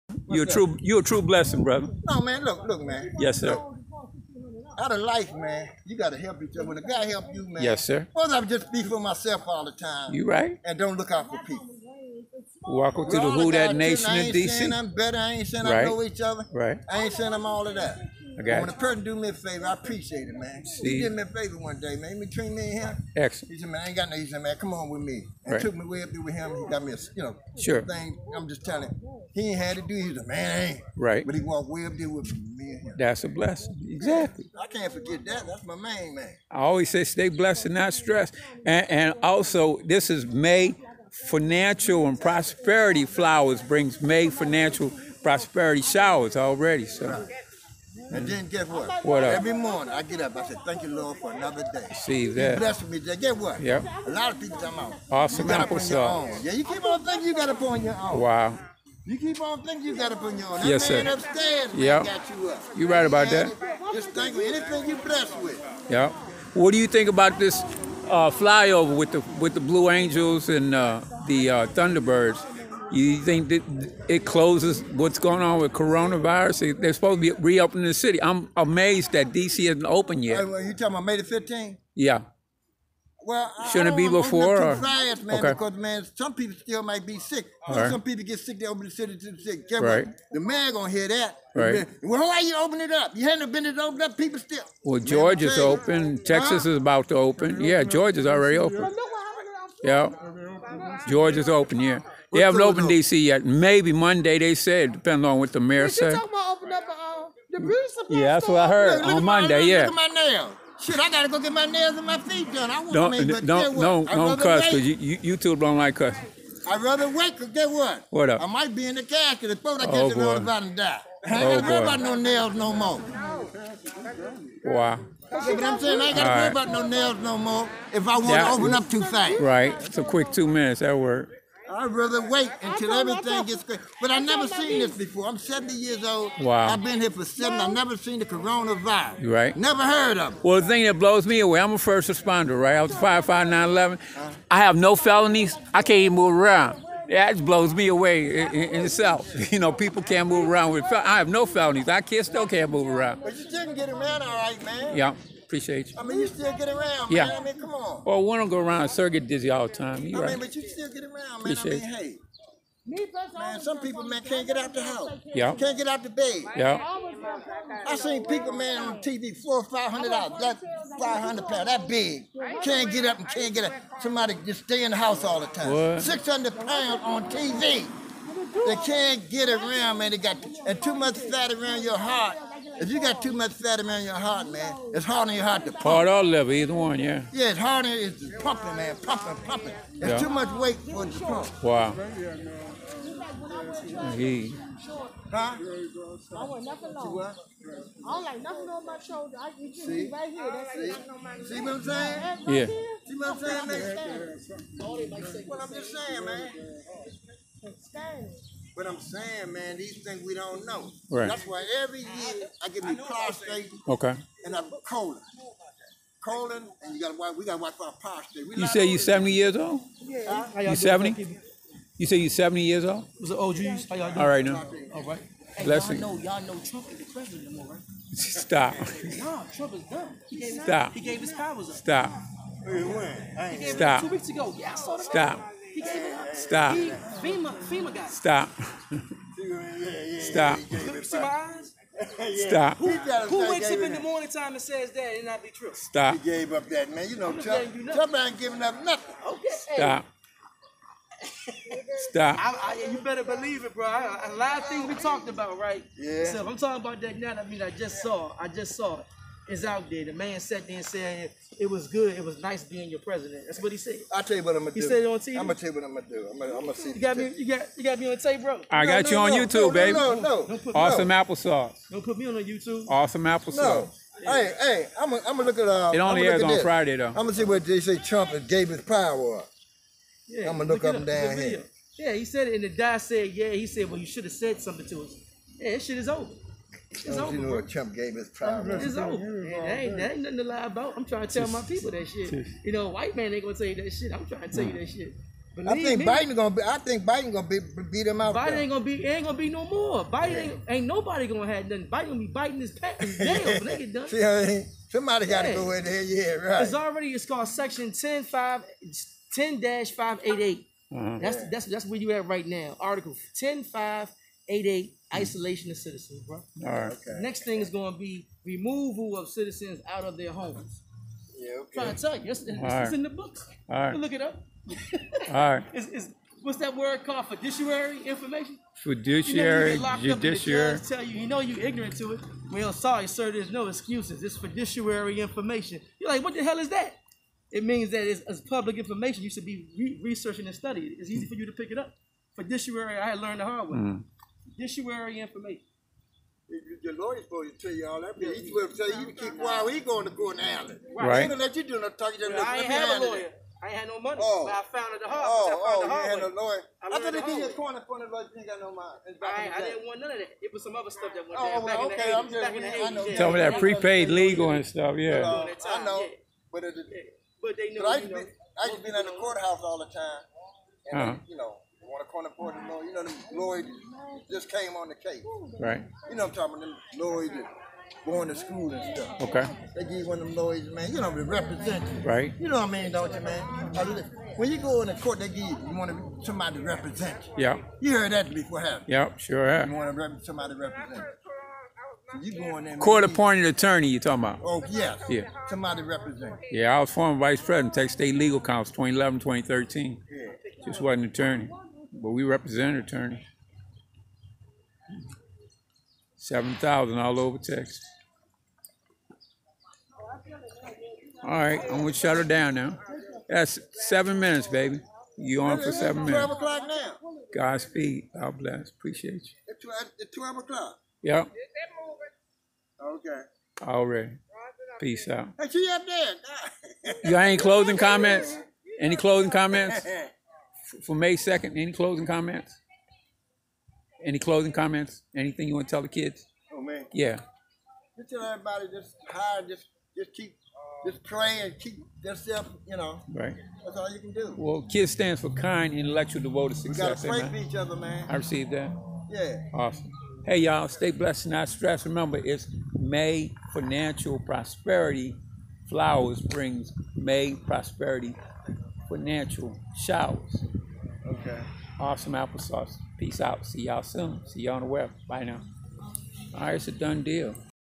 Reopening1st Saturday of MAY With RevMarkPinman with Fellow Military Veterans of theDCDREAM Center
Fresh Air@DCDREAMCENTER Message FromFellow Veteran of the WHOD'ATNATION OF WASHINGTON DC Being ABlessing to the Sr.Citizens n Returning a Favor all thru theMIGHTY NAMEOFJESUS Amen,RevMARK Pinman Perez Rolling out of DA-BOX  yet notout to lunch wellnot yet anyways,almost rolling out of the Woods, though PRAISE GOD Showers brings MAY PROSPERITY Financial Flowers MAY Brings Love in the Sun thru TheHOLY SPIRIT ABOVE,Amen, Aaamen N AAAMEN!